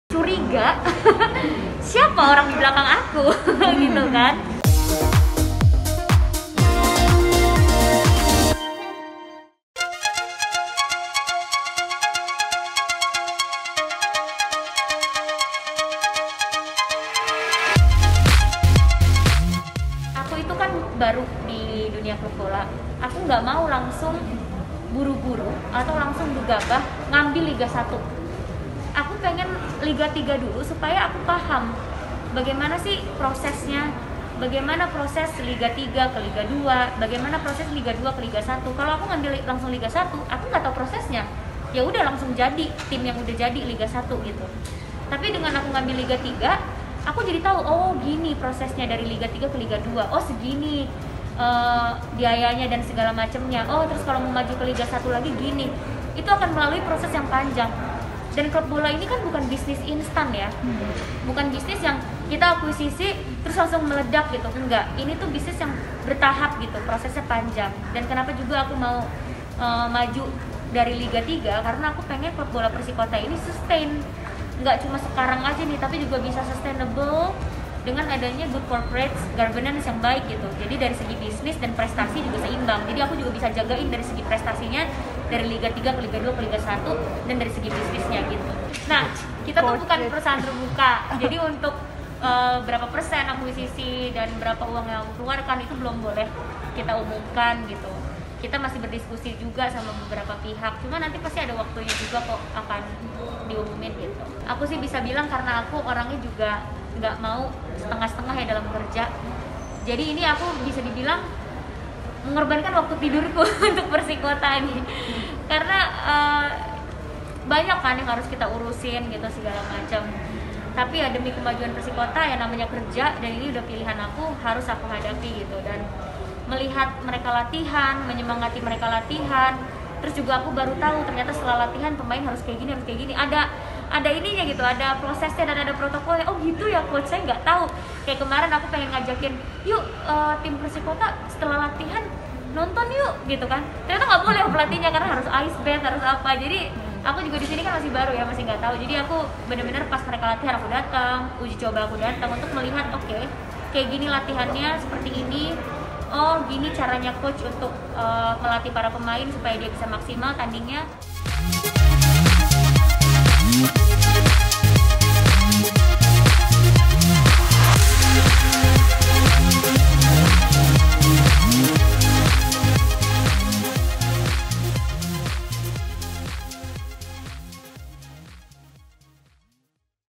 Jadi 'Ibu Ajaib', Prilly Latuconsina Korbankan Waktu Tidur Demi Persikota
KAPANLAGI.COM - KLovers, Memiliki dan mengelola klub sepakbola menjadi hal baru bagi aktris 25 tahun, Prilly Latuconsina. Banyak hal baru yang harus dipelajari melalui diskusi-diskusi panjang. Namun, demi memajukan Persikota Tangerang, Ily rela korbankan waktu tidurnya.